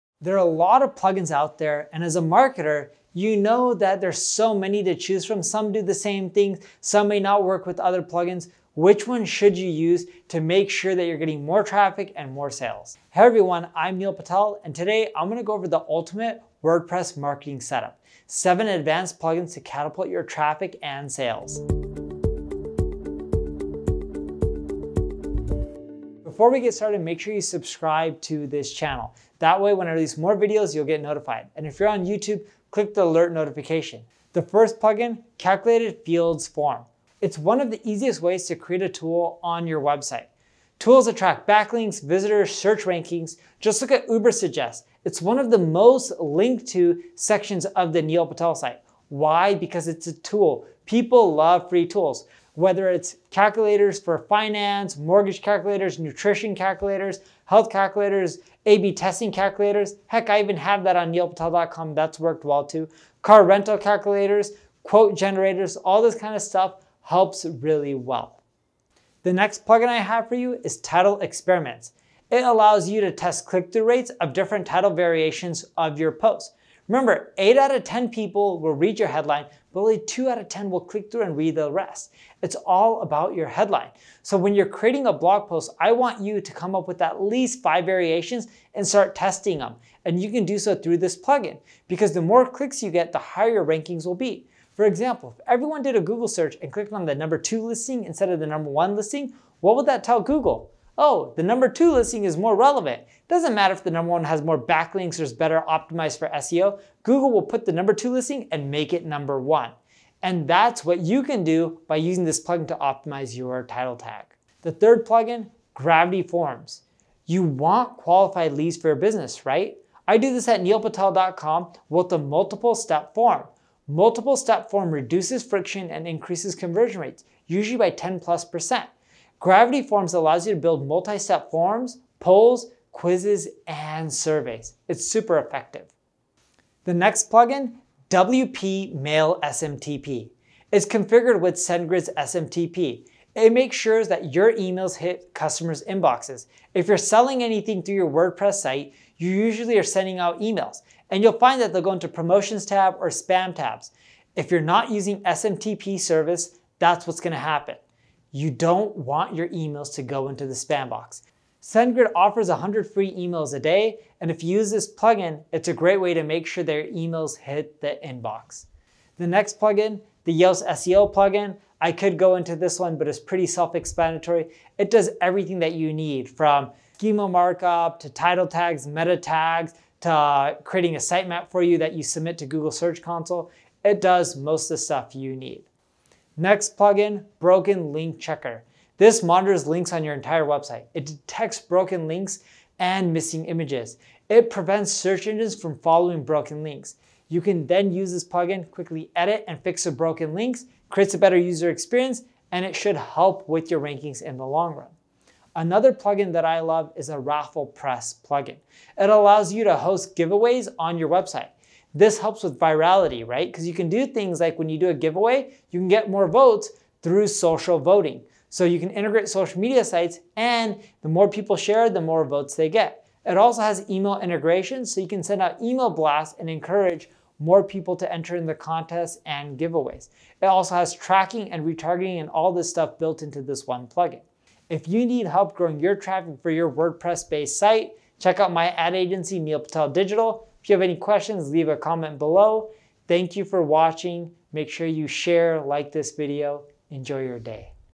The Ultimate Wordpress Marketing Setup: 7 Advanced Plugins to Catapult Traffic and Sales
The Ultimate Wordpress Marketing Setup: 7 Advanced Plugins to Catapult Traffic and Sales // There are a lot of plugins out there, and as a marketer you know that there's so many to choose from. Some do the same thing, some may not work with other plugins. Which one should you use to make sure that you're getting more traffic, and more sales? Today I'm going to go over The Ultimate WordPress Marketing Setup; Seven Advanced Plugins to Catapult Your Traffic and Sales

The first plugin, Calculated Fields Form. It's one of the easiest ways to create a tool on your website. Tools attract backlinks, visitors, search rankings, just like a Ubersuggest. It's one of the most linked-to sections of the Neil Patel site. Why? Because it's a tool.

People love free tools, whether it's calculators for finance, mortgage calculators, nutrition calculators, health calculators, A/B testing calculators.

The next plugin I have for you is Title Experiments. It allows you to test click through rates of different title variations of your posts. Remember, eight out of 10 people will read your headline, but only two out of 10 will click through, and read the rest.

It's all about your headline, so when you're creating a blog post, I want you to come up with at least five variations, and start testing them, and you can do so through this plugin, because the more clicks you get, the higher your rankings will be.

The third plugin Gravity Forms. You want qualified leads for your business, right? I do this at neilpatel.com with a multiple step form. Multiple step form reduces friction, and increases conversion rates usually by 10 plus percent. Gravity Forms allows you to build multi step forms, polls, quizzes and surveys. It's super effective.

The next plugin, WP Mail SMTP. It's configured with SendGrid's SMTP. It makes sure that your emails hit customer's inboxes. If you're selling anything through your WordPress site, you usually are sending out emails, and you'll find that they're going to Promotions tab, or Spam tabs.

SendGrid offers a hundred free emails a day, and if you use this plugin, it's a great way to make sure their emails hit their inbox.

The next plugin, the Yoast SEO plugin. I could go into this one, but it's pretty self explanatory. It does everything that you need from markup to title tags, meta tags, to creating a site map for you that you submit to Google search console. It does most of the stuff you need.

Next plugin, Broken Link Checker. This monitors links on your entire website. It detects broken links, and missing images. It prevents search engines from following broken links.

You can then use this plugin to quickly edit, and fix the broken links, create a better user experience, and it should help with your rankings in the long run.

Another plugin that I love is a RafflePress plugin. This helps with virality, right? Because you can do things like when you do a giveaway, you can get more votes through social voting, so you can integrate social media sites, and the more people share it the more votes they get.

It also has email integration, so you can send out email blast, and encourage more people to enter in the contests, and giveaways. It also has tracking and retargeting, and all this stuff built into this one plugin.